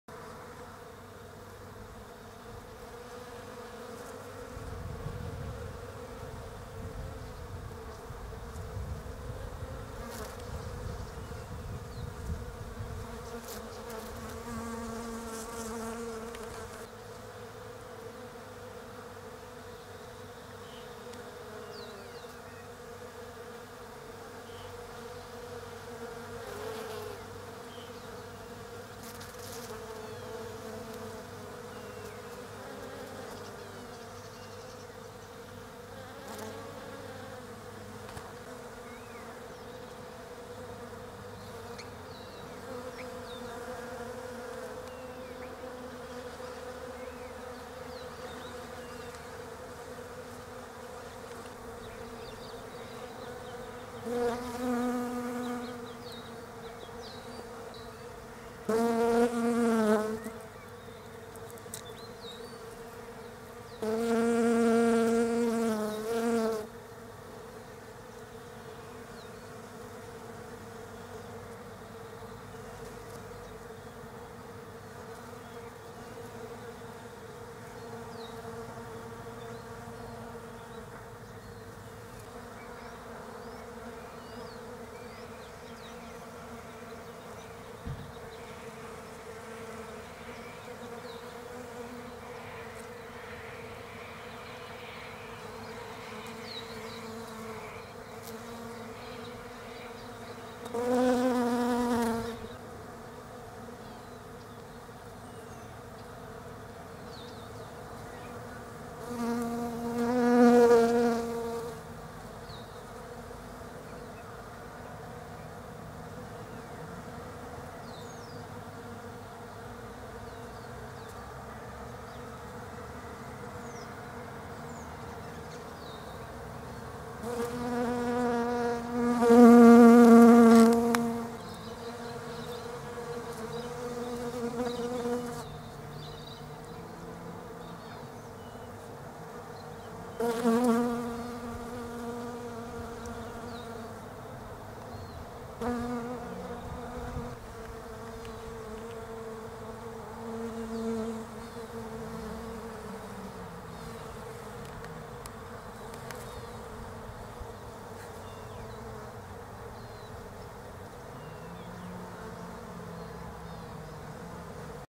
Bees in Desert Ash Tree ❀ '◕◕'))))=
Here are some European bees buzzing about the Desert Ash (Fraxinus oxycarpa) flower things in autumn down under. When there are not too many other blooms out, these unusual looking flowers always attract the bees on the otherwise bare-branched tree. Funny how the Desert Ash blooms in autumn/winter instead of spring like other deciduous trees. I hope you like the buzzing sound and that it relaxes you and brings you back to nature, again. (Down to earth!)